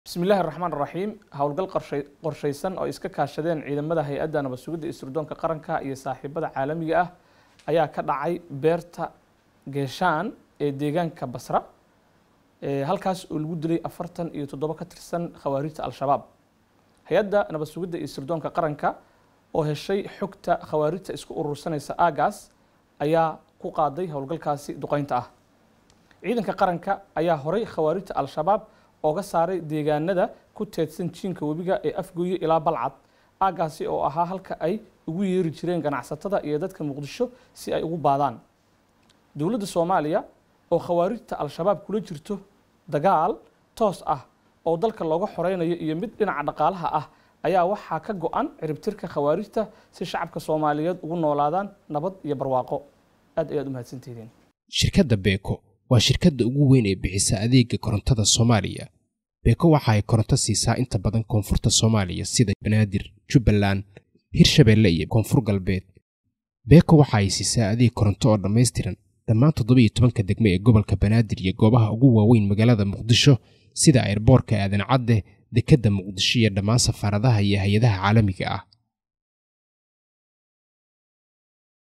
Ciidanka Hay'adda NISA oo kaashanaya saaxiibada Caalamka ayaa Basra ku dilay 47 ka tirsan Al shabaab
Ciidanka Hay'adda NISA oo kaashanaya saaxiibada Caalamka ayaa Beeraha Geeshaan ee deegaanka Basra ku dilay 47 ka tirsan Al shabaab.